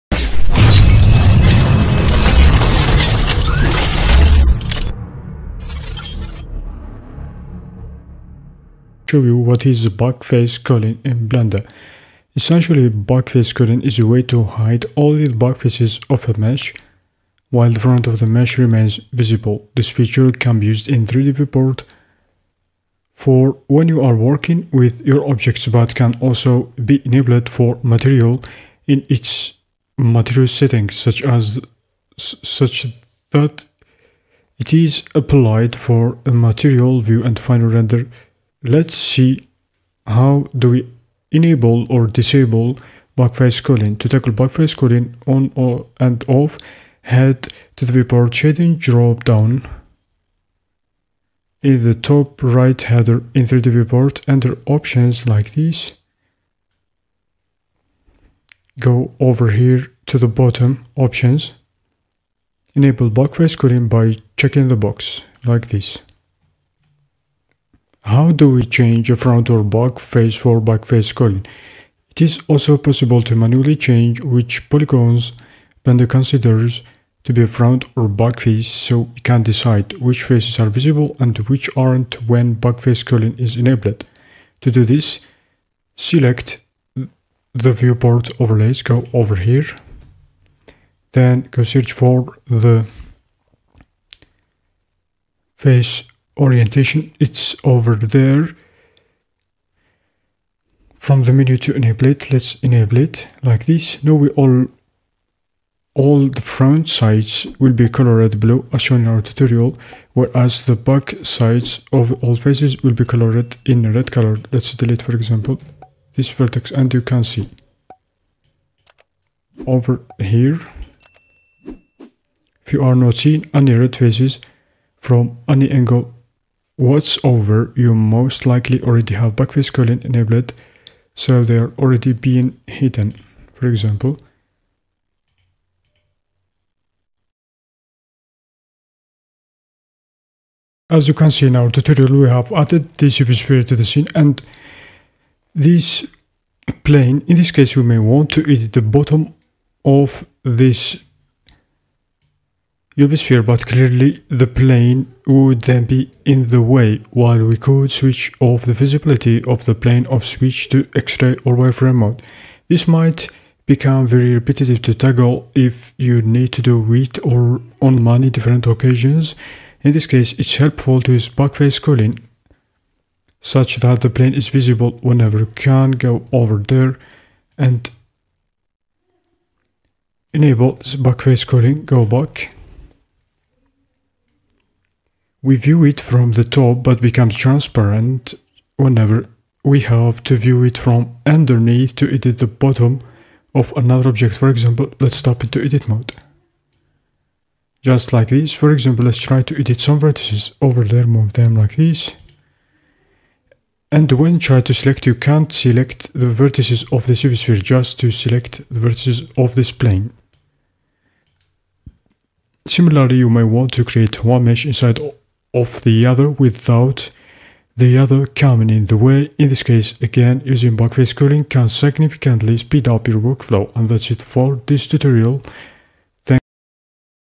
What is backface culling and when is it useful Blender ?
Learn about the importance of backface culling in Blender and how it can improve your 3D modeling and rendering workflow. In this video, we'll dive into what backface culling is, when it's useful, and how it can help you optimize your models and scenes for faster rendering and better performance. Whether you're a beginner or an experienced Blender user, this tutorial will help you understand the benefits of backface culling and how to apply it to your projects.

Backface culling is a crucial technique in 3D modeling and rendering, especially in Blender. But what exactly is it? Simply put, backface culling is the process of ignoring the polygons of a 3D object that are facing away from the camera. This means that only the visible surfaces are rendered, helping optimize performance and improve rendering speed.

So, when is backface culling useful in Blender? Imagine creating a detailed environment with lots of objects. If Blender continues to render every single polygon, it can slow down your workflow. By enabling backface culling, you streamline the rendering process, allowing you to focus on what really matters—the visible parts of your scene.

Additionally, backface culling can be beneficial when working with complex materials and shaders. It helps you efficiently manage the rendering of overlapping objects, ensuring that only the necessary faces are processed. This not only saves time but also enhances the overall performance of your project.

In summary, backface culling is an essential tool in Blender that optimizes rendering by discarding unseen polygons. Whether you're working on a game, an animation, or a static render, understanding and utilizing this feature can make a significant difference in your workflow.